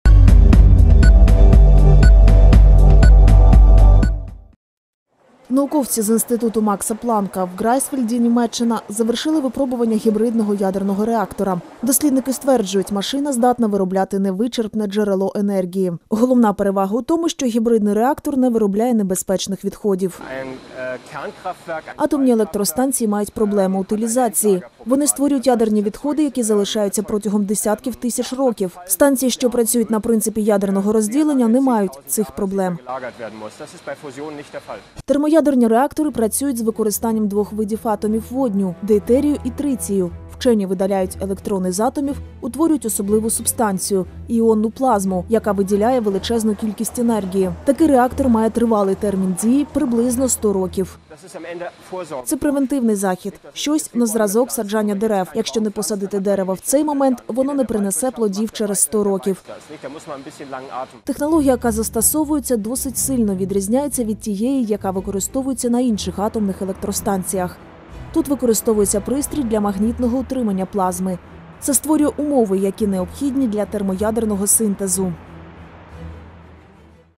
Гібридний ядерний реактор. Вчені знайшли невичерпне джерело енергії
Науковці з Інституту Макса Планка у Грайфсвальді (Німеччина) завершили випробування гібридного ядерного реактора. Дослідники стверджують: машина здатна виробляти невичерпне джерело енергії. Головна перевага у тому, що гібрідний реактор не виробляє небезпечних відходів.

Термоядерні реактори працюють з використанням двох видів атомів водню: дейтерію і тритію. Вчені видаляють електрони з атомів, утворюють особливу субстанцію – іонну плазму, яка виділяє величезну кількість енергії. Такий реактор має тривалий термін дії - приблизно сто років.